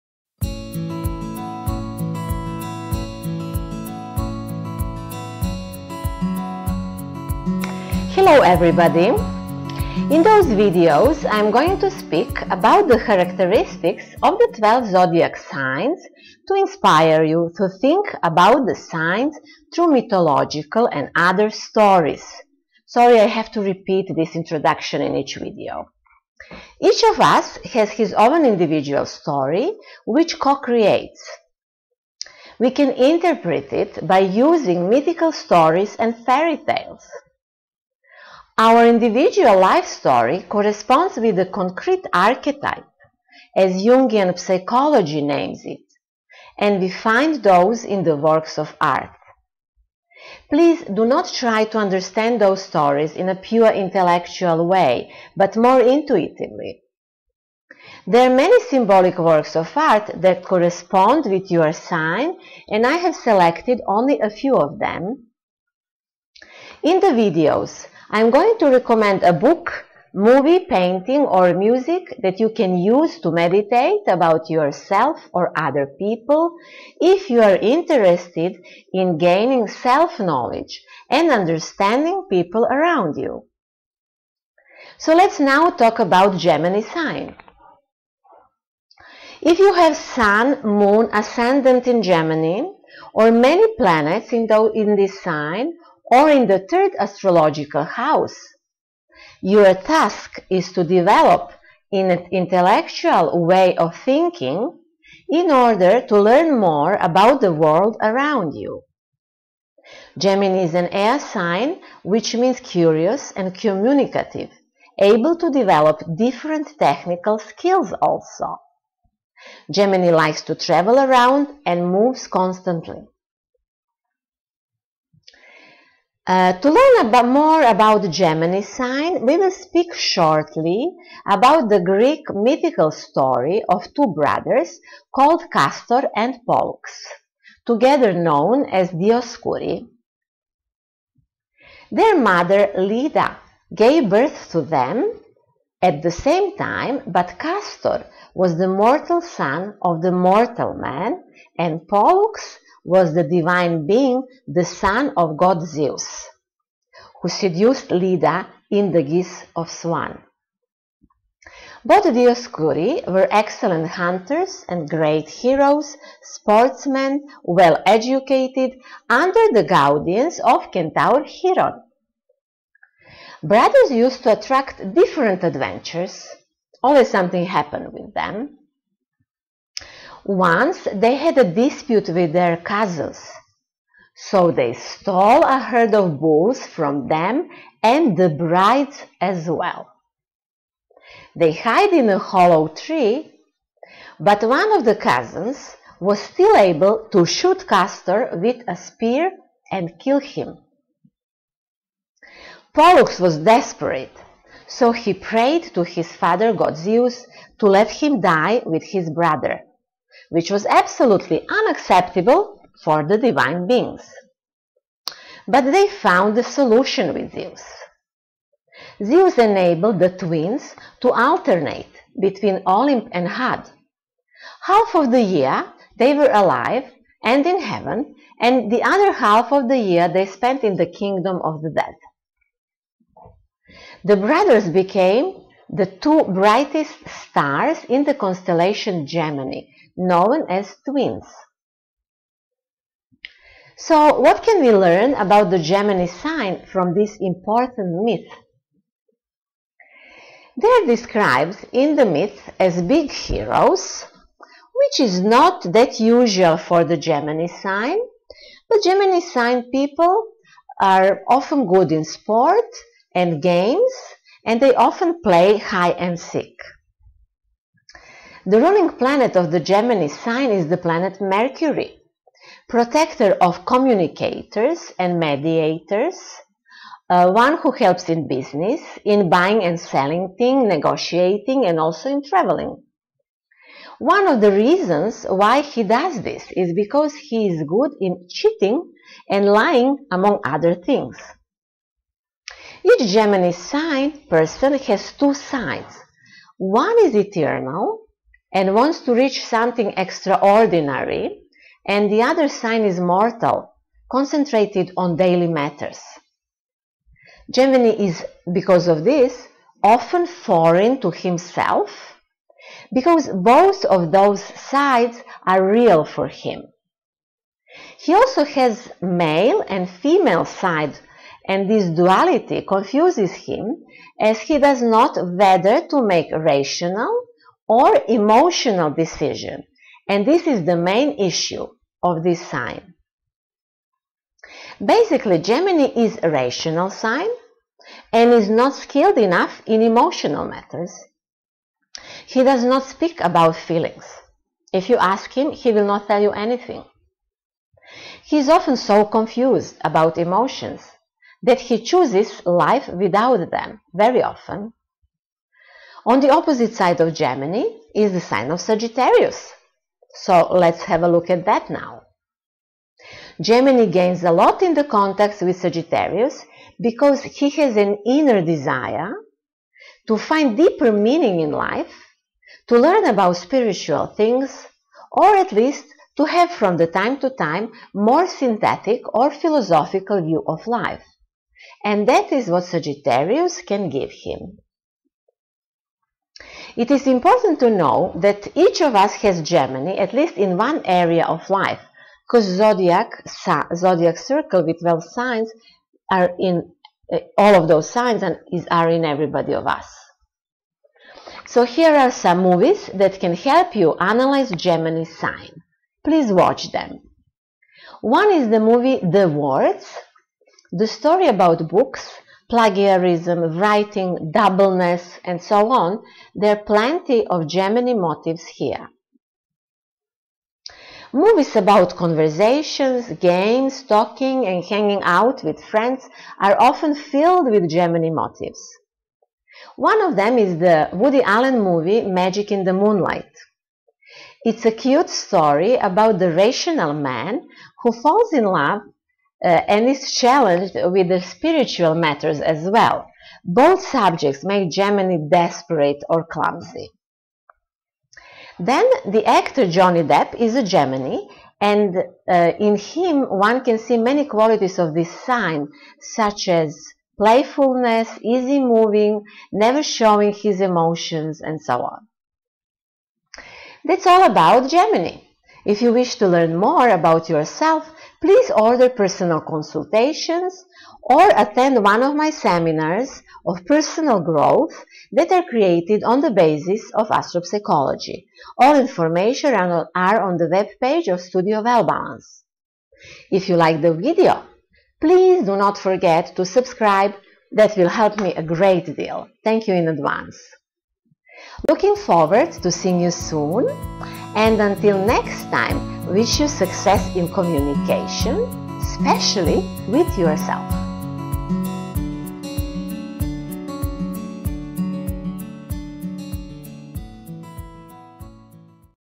Astro-psychology: Gemini myth and personality
In this video I will be speaking about the characteristics of a concrete zodiac sign to inspire you to think about it through mythological and other stories. Please do not try to understand these stories only in a pure intellectual way but more intuitively.
Mgr. Tatjana Micić Ph.D.